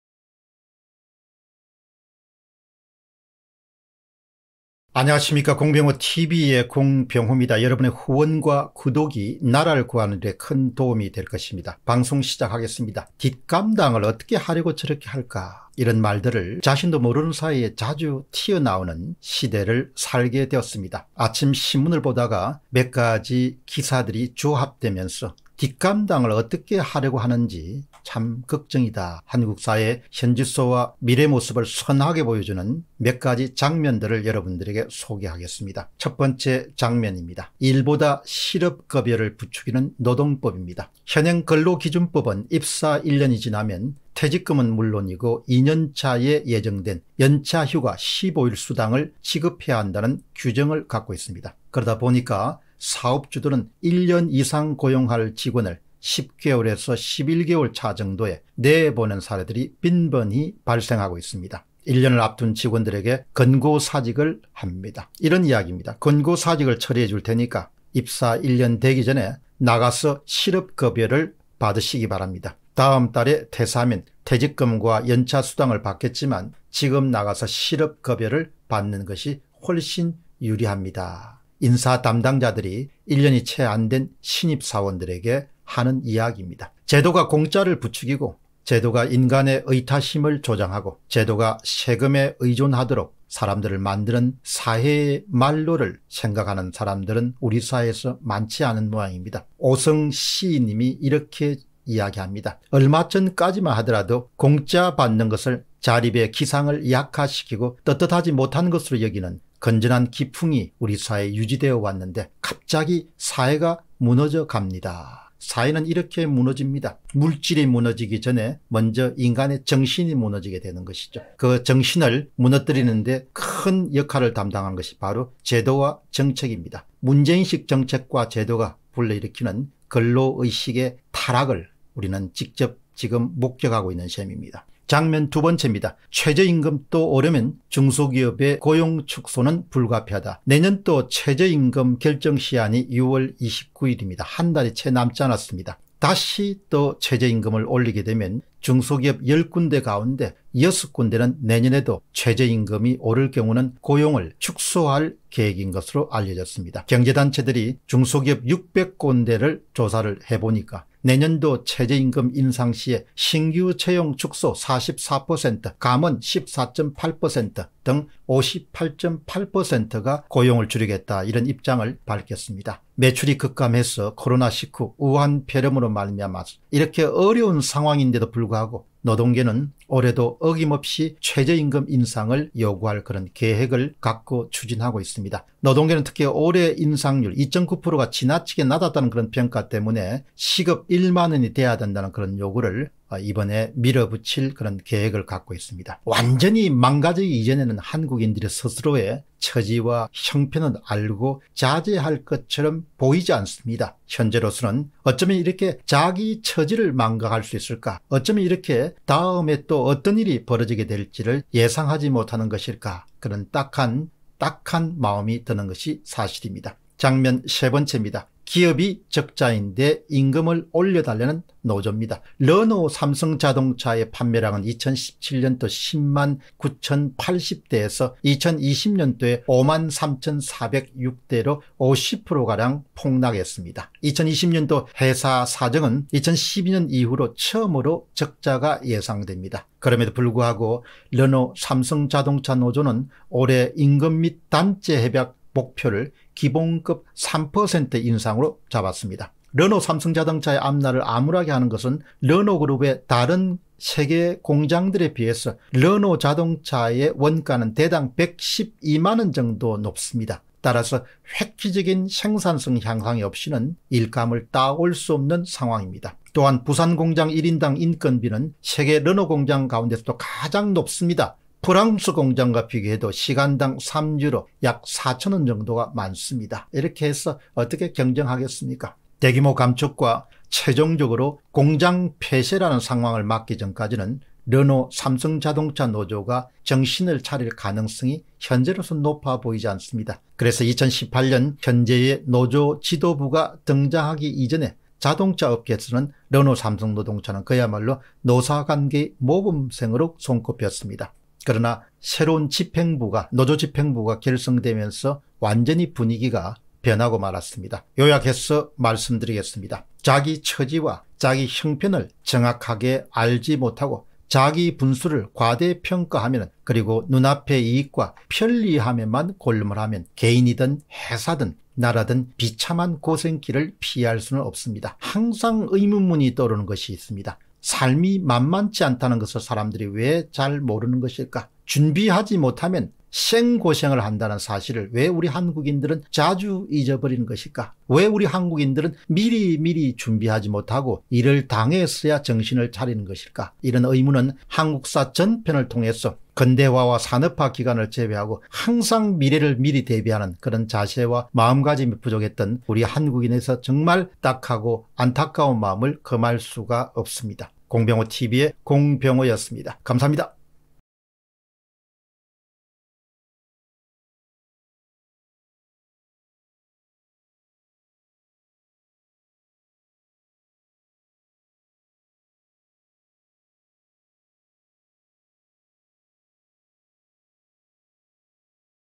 정신 차리지 못하면 / 우리를 기다리는 것 [공병호TV]
Bitcoin : 3MTW849AkP2ftLeSK2teZHMmzLu7Z83s9U

참고자료
1. "일 하느니 차라리 실업급여"…꼼수 부추기는 노동법, 백승현, 한국경제, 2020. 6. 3
2. [사설] "최저임금 또 오르면 中企 60% 고용 축소" 현실 직시해야, 한국경제, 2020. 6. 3
3. 적자 뻔한데…월급 올려달라는 르노삼성 노조, 도병욱, 한국경제, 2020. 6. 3